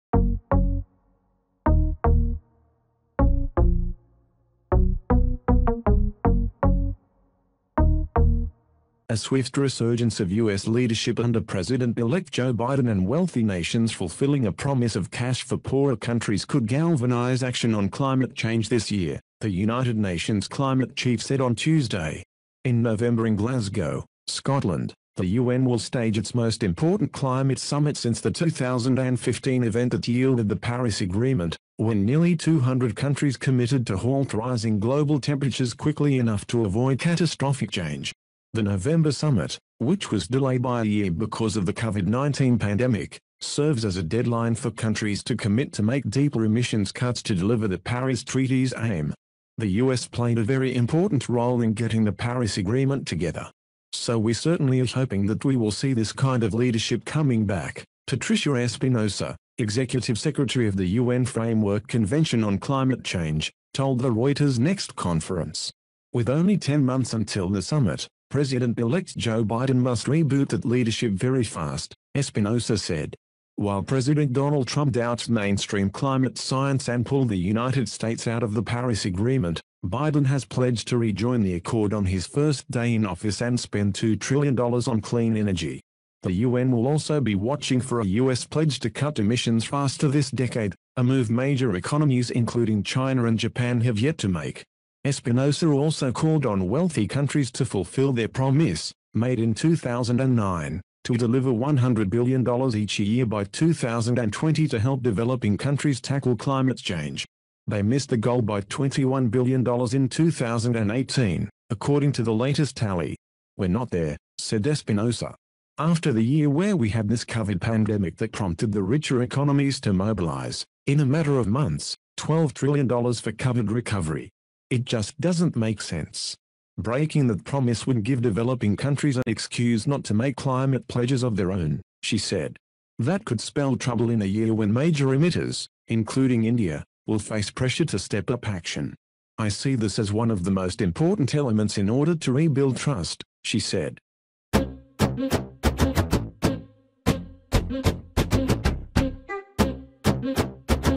U.N. climate chief looks to Biden to boost global action
A swift resurgence of U.S. leadership under President-elect Joe Biden and wealthy nations fulfilling a promise of cash for poorer countries could galvanise action on climate change this year, the United Nations climate chief said on Tuesday.

In November in Glasgow, Scotland, the U.N. will stage its most important climate summit since the 2015 event that yielded the Paris Agreement, when nearly 200 countries committed to halt rising global temperatures quickly enough to avoid catastrophic change.
The November summit, which was delayed by a year because of the COVID-19 pandemic, serves as a deadline for countries to commit to make deeper emissions cuts to deliver the Paris treaty’s aim.
“The U.S. played a very important role in getting the Paris Agreement together. So we certainly are hoping that we will see this kind of leadership coming back,” Patricia Espinosa, executive secretary of the U.N. Framework Convention on Climate Change, told the Reuters Next conference.
With only 10 months until the summit, President-elect Joe Biden must reboot that leadership “very fast”, Espinosa said.
While President Donald Trump doubts mainstream climate science and pulled the United States out of the Paris Agreement, Biden has pledged to re-join the accord on his first day in office and spend $2 trillion on clean energy.
The U.N. will also be watching for a U.S. pledge to cut emissions faster this decade - a move major economies including China and Japan have yet to make.
Espinosa also called on wealthy countries to fulfil their promise, made in 2009, to deliver $100 billion each year by 2020 to help developing countries tackle climate change. They missed the goal by $21 billion in 2018, according to the latest tally.
“We’re not there,” said Espinosa. “After the year where we had this COVID pandemic that prompted the richer economies to mobilise, in a matter of months, $12 trillion for COVID recovery. It just doesn’t make sense.”
Breaking that promise would give developing countries an excuse not to make climate pledges of their own, she said. That could spell trouble in a year when major emitters, including India, will face pressure to step up action.
“I see this as one of the most important elements in order to rebuild trust,” she said.